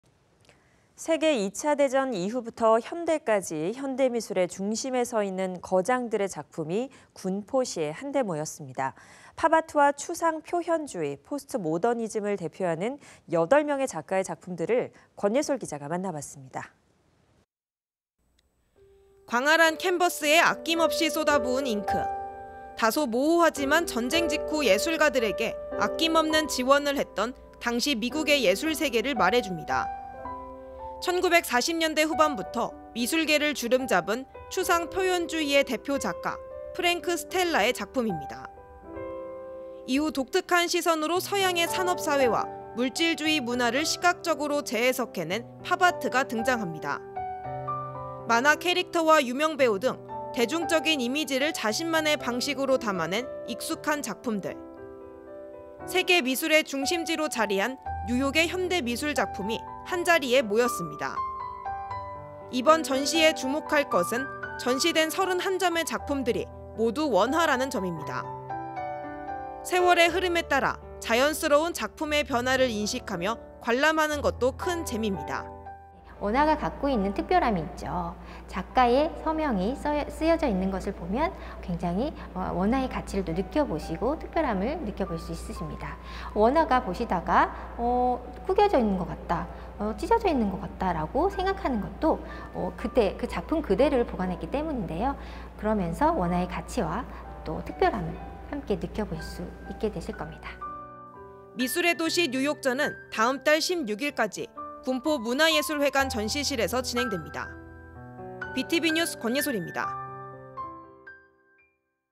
[군포] 한데 모인 현대미술 '미술의 도시 뉴욕'
[앵커멘트]

세계 2차 대전 이후부터 현대까지
현대 미술의 중심에 서있는 거장들의 작품이
군포시에 한 데 모였습니다.

팝아트와 추상표현주의, 포스트모더니즘을 대표하는
8명의 작가 작품들을
권예솔 기자가 만나봤습니다.

[리포트]

광활한 캔버스에 아낌없이 쏟아부은 잉크.

다소 모호하지만 전쟁 직후 예술가들에게
아낌없는 지원을 했던
당시 미국의 예술 세계를 말해줍니다.

1940년대 후반부터 미술계를 주름잡은
추상표현주의의 대표 작가,
프랭크 스텔라의 작품입니다.

이후 독특한 시선으로 서양의 산업사회와
물질주의 문화를 시각적으로 재해석해 낸
팝아트가 등장합니다.

만화 캐릭터와 유명 배우 등
대중적인 이미지를 자신만의 방식으로 담아낸 익숙한 작품들.

세계 미술의 중심지로 자리한
뉴욕의 현대 미술 작품이 한자리에 모였습니다.

이번 전시의 주목할 것은
전시된 37점의 작품들이 전부 원화라는 점입니다.

세월의 흐름에 따라
자연스러운 작품의 변화를 인식하며
관람하는 것도 큰 재미입니다.

[김숙현 / 도슨트]
"원화가 갖고 있는 특별함이 있죠.
작가의 서명이 쓰여 있는 것을 보면
원화의 가치를 느껴보시고 특별함을 느껴보실 수 있으십니다.
보시다가 (작품이) 구겨져있는 것 같다, 찢어져있는 것 같다고
생각하는 것도 작품 그대로를 보관했기 때문인데요.
원화의 가치와 특별함을 느껴보실 수 있게 되실 겁니다.”

'미술의 도시 뉴욕'전은 다음달 16일까지
군포문화예술회관 전시실에서 진행됩니다.

Btv뉴스 권예솔입니다.

촬영/편집:김호석 기자